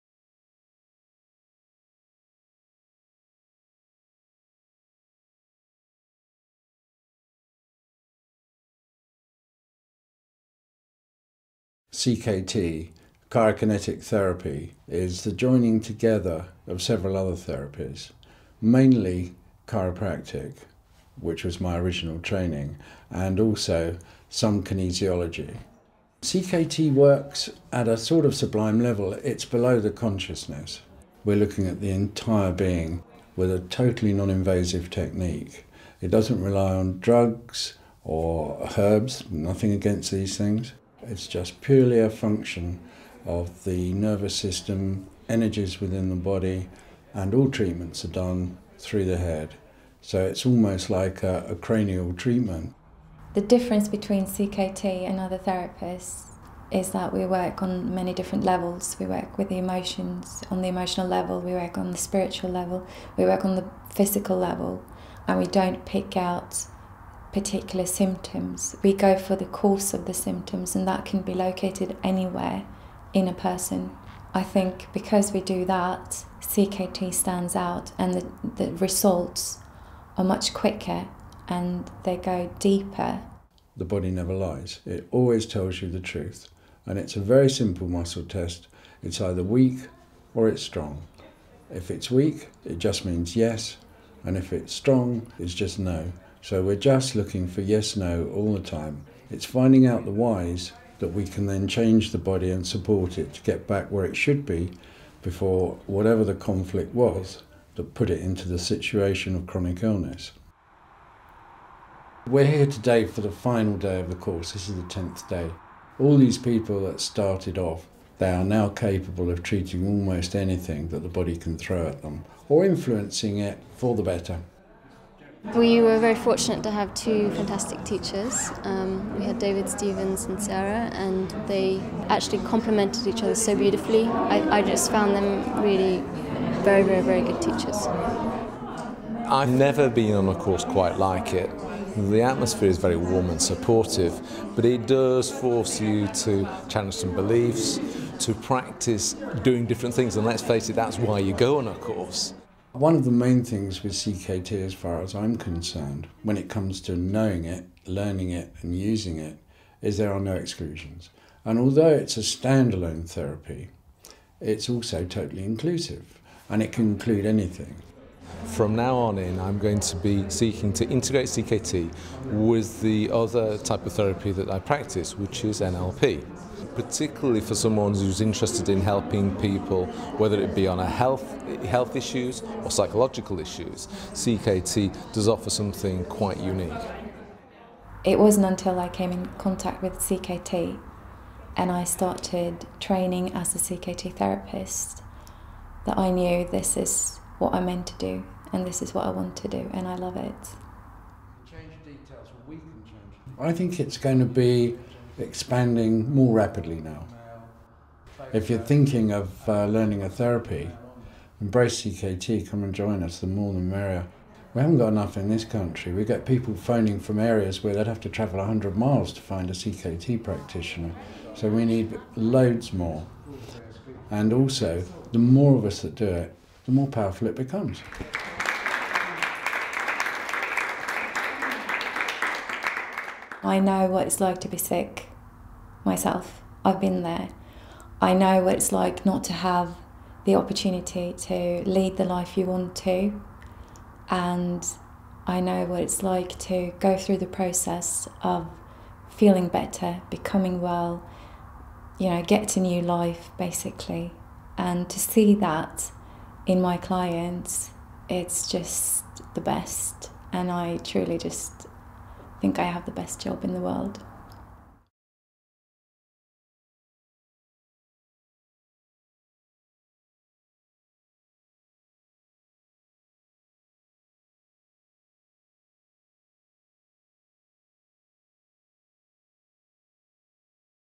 Allergy Treatment - Chirokinetic Therapy (CKT)
Chirokinetic Therapy (CKT) is an outstanding treatment, for many of today's ills. It aims to balance the body's physical, emotional and spiritual needs allowing harmony to be restored. It works by correcting imbalances and redirecting energy to depleted areas.